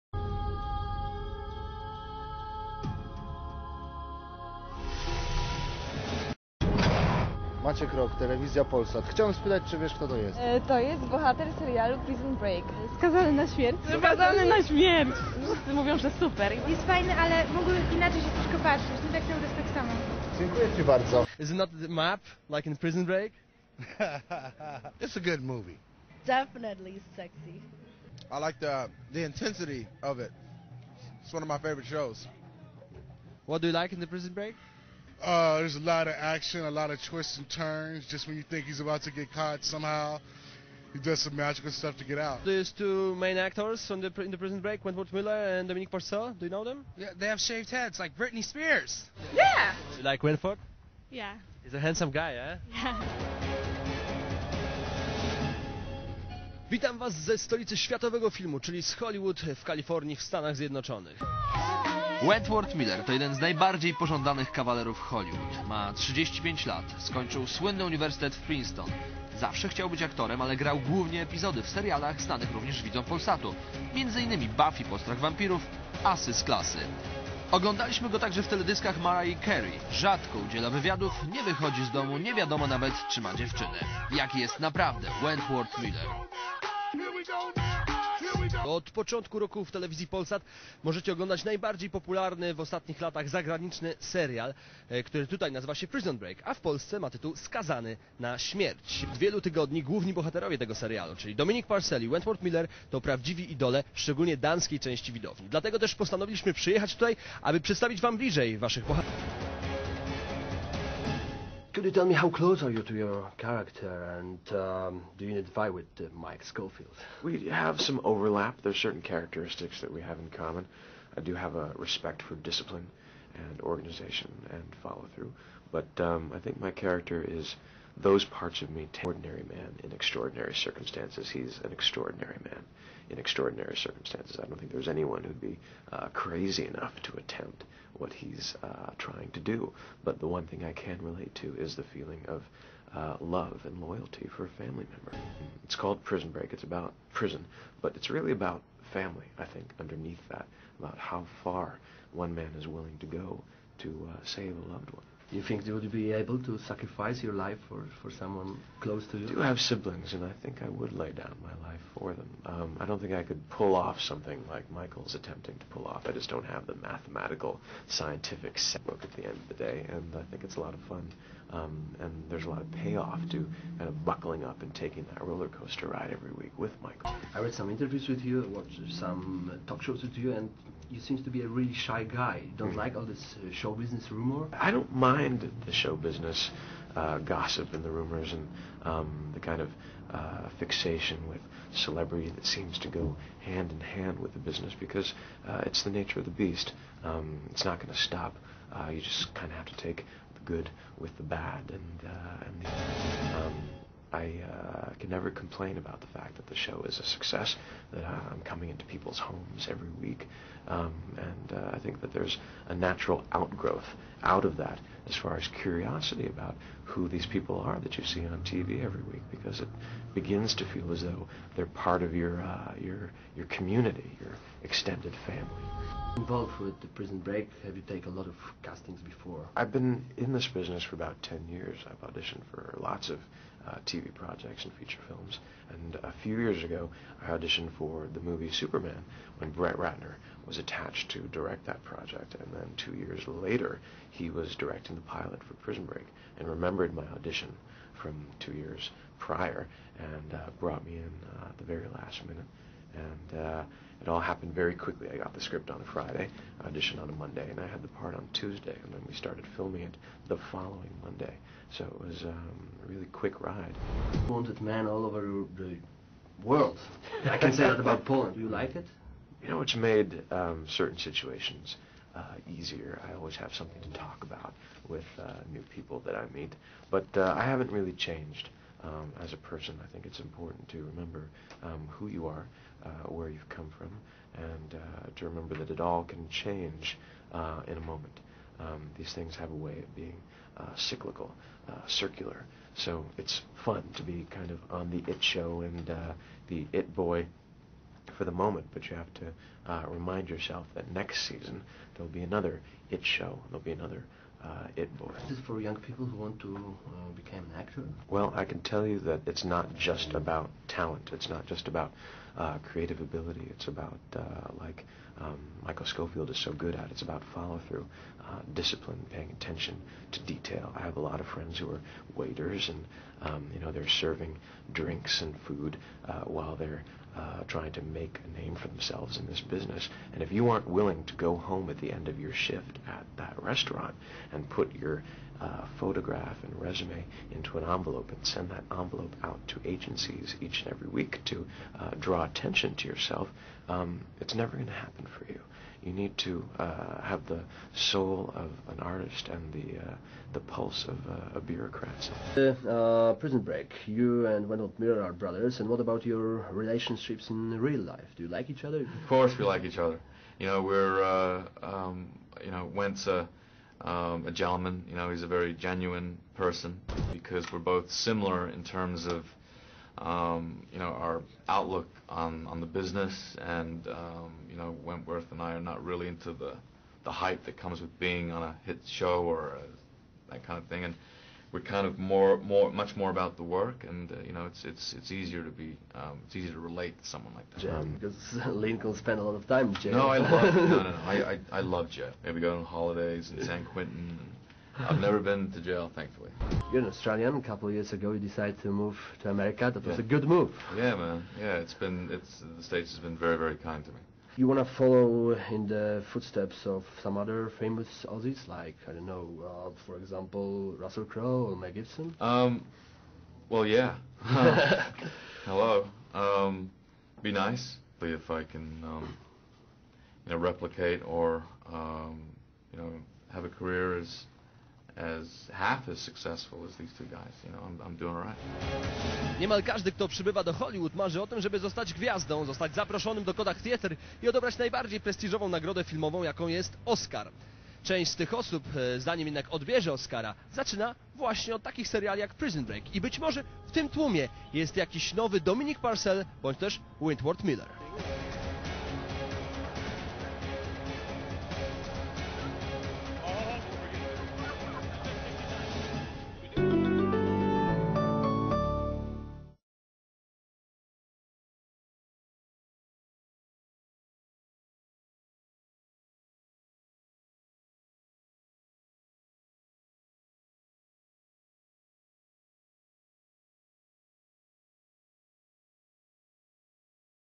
Wywiad Skazany na Śmierć (Prison Break) PL
Film z polskim wywiadem dla TV Polsat zmontowany przez Grupę Kanału Youtube Prison Break